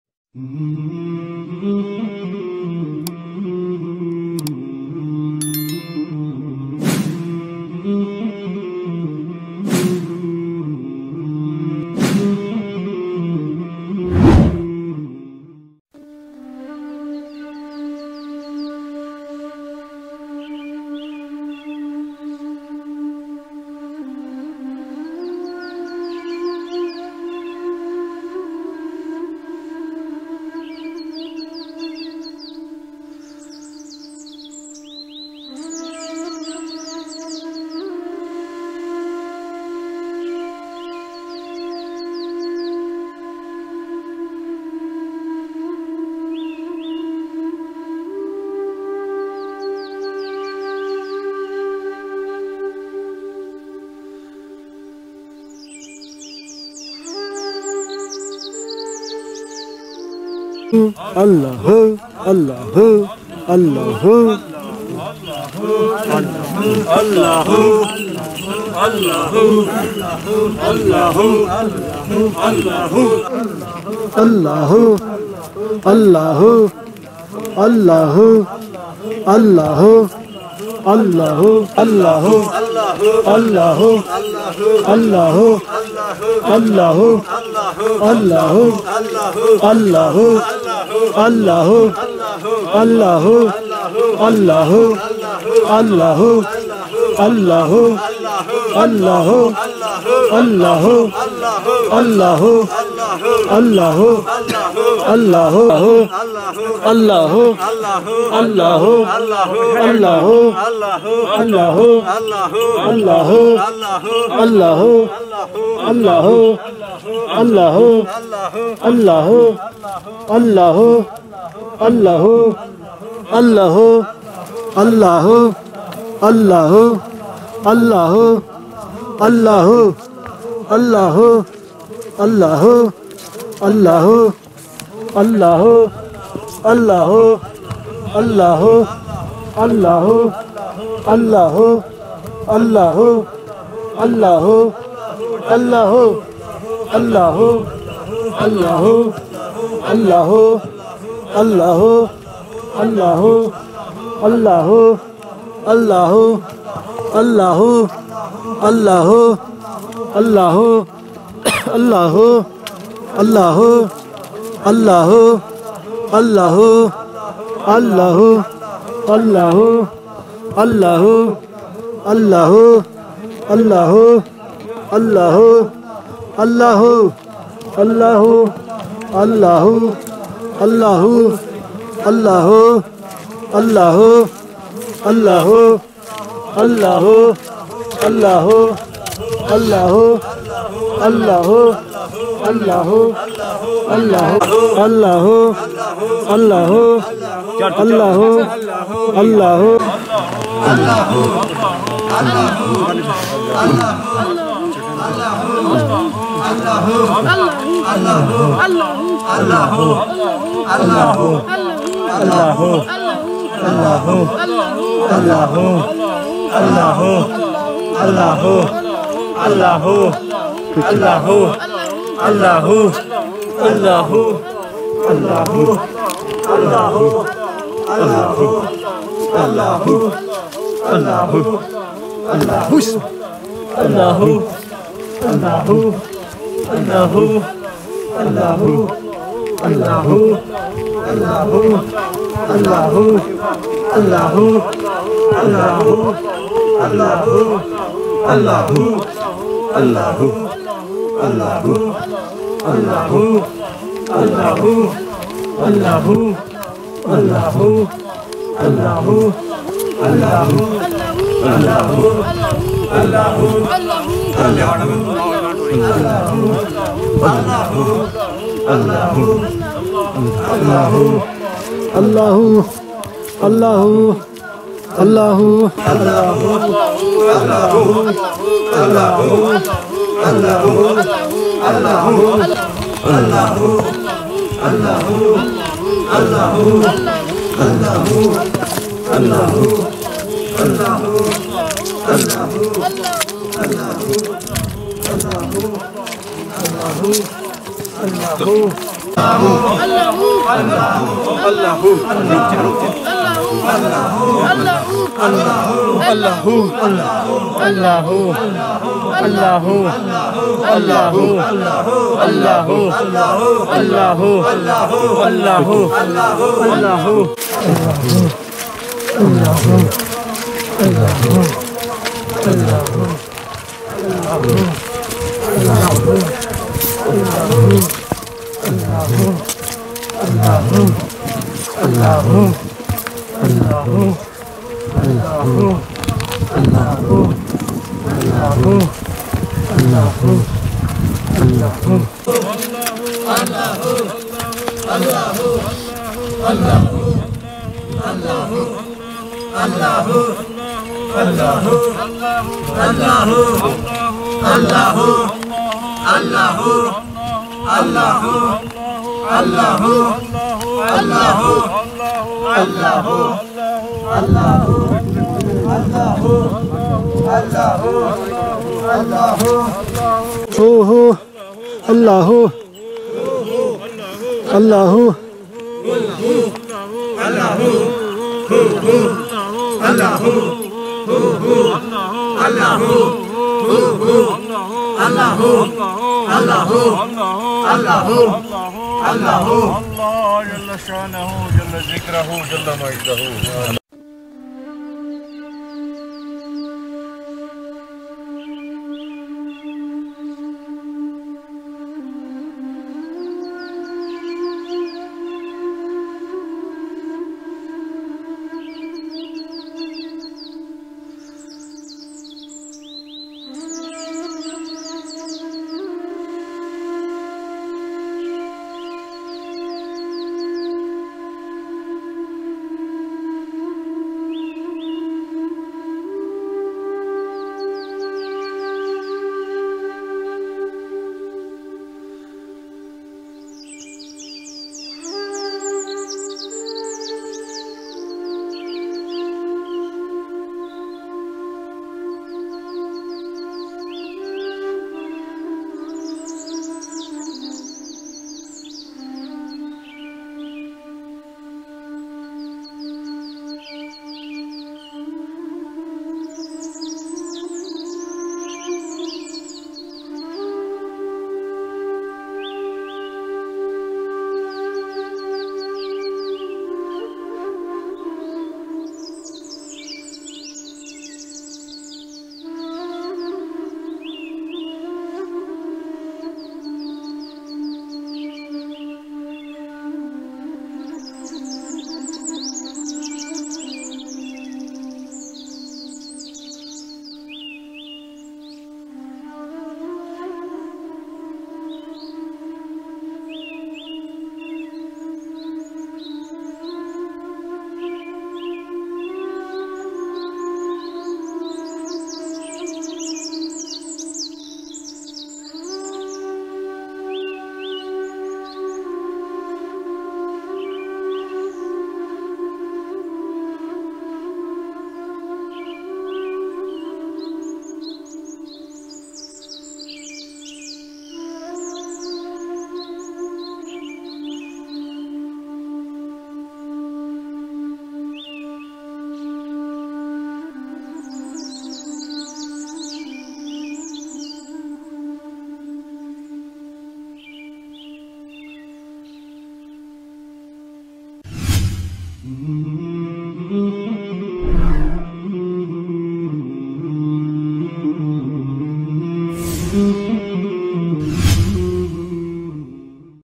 Ziker Allah Hu in deep forests of Kashmir
Syed Farhat Andrabi with his disciples and doing Ziker Allah Hu in deep forests of Kashmir.
August 2022